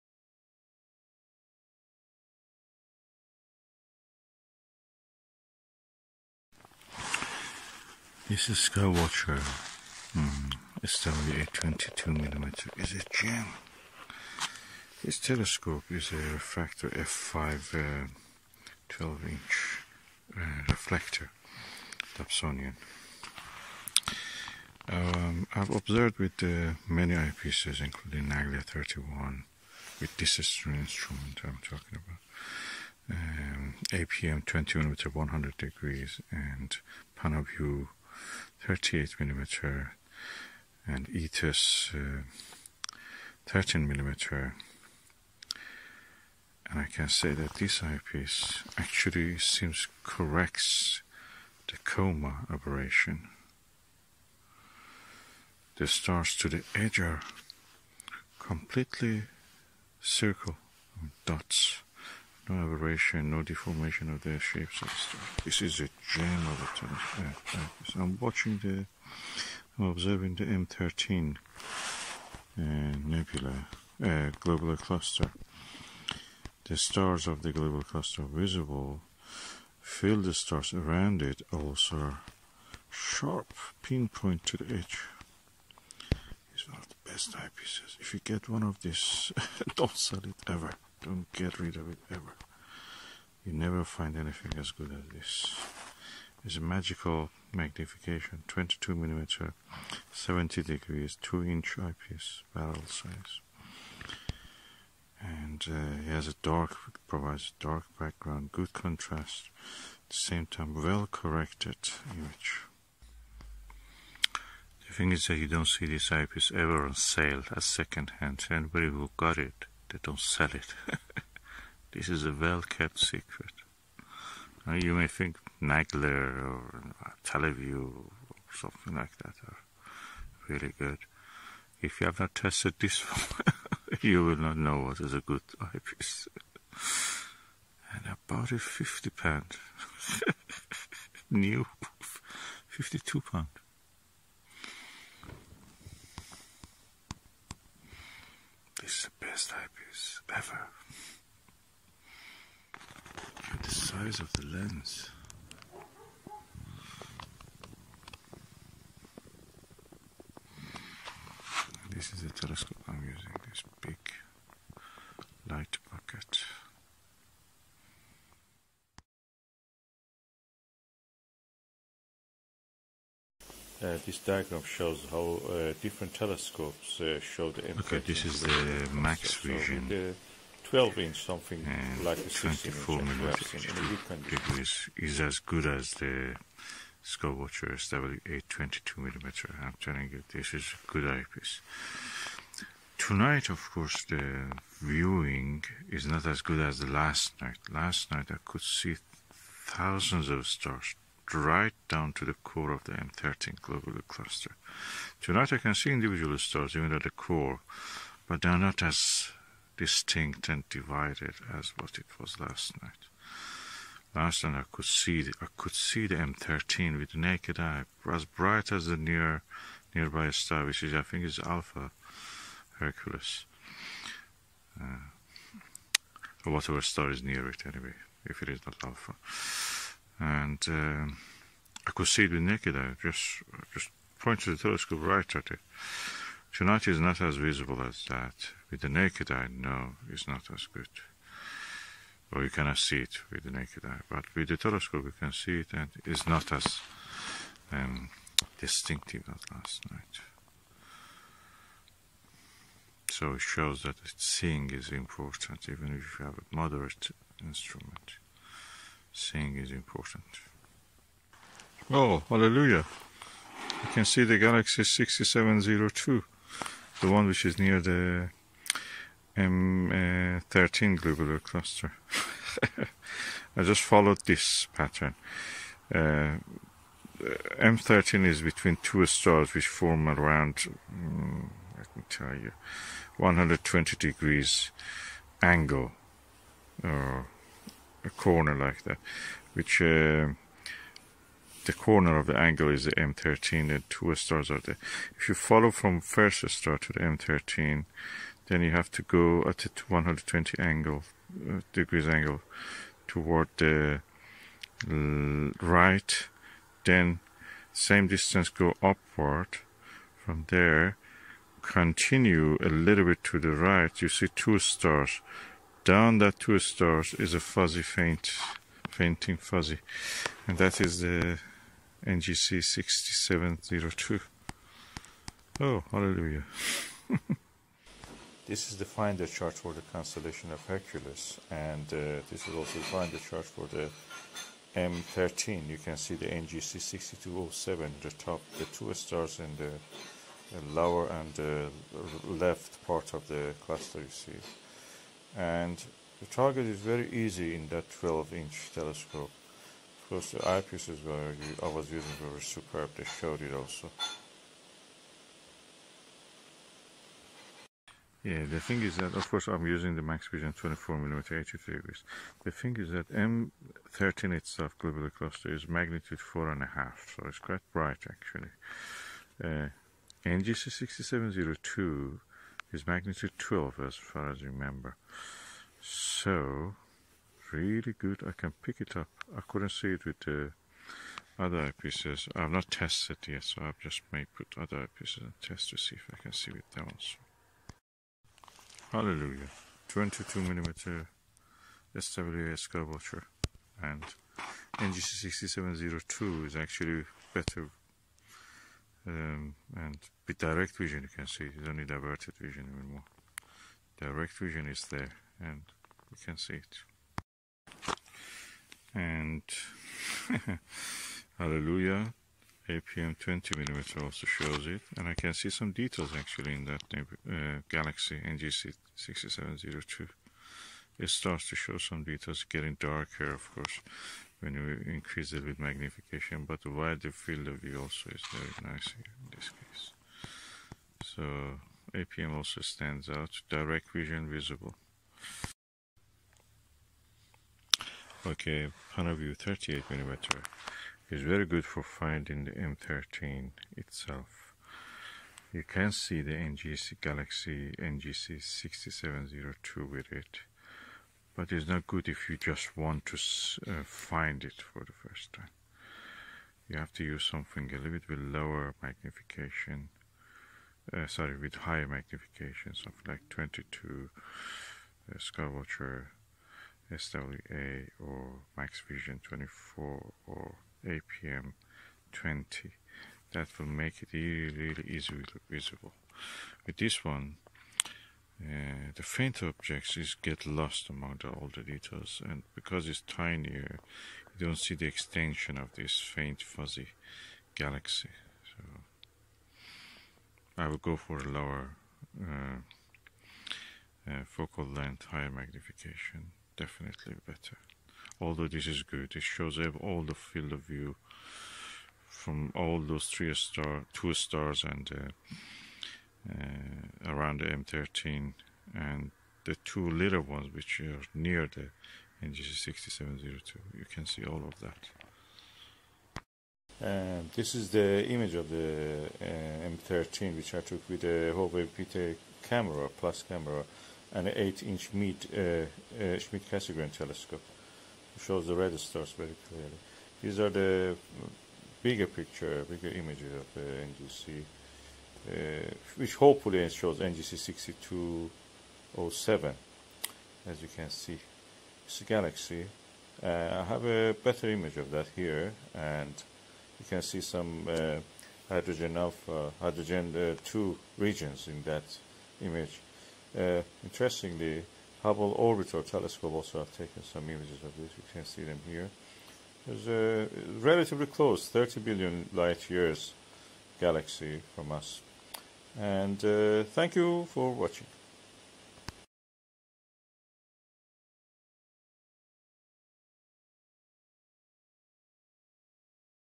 Observing Galaxy NGC 2607, M13 Globular/Skywatcher Skyliner 300P Flextube Dobsonian Telescope
In this video I have used these telescope and eyepieces:
Skywatcher Skyliner 300P Flextube Dobsonian 12 Inch Telescope.
Skywatcher SWA 22mm 70 Degree eyepiece.
Panaview 38mm 70 Degrees eyepiece.
APM HDC 20mm 100 Degrees Ultra Wide Angle eyepiece.
Maxvision 24mm 82 Degrees eyepie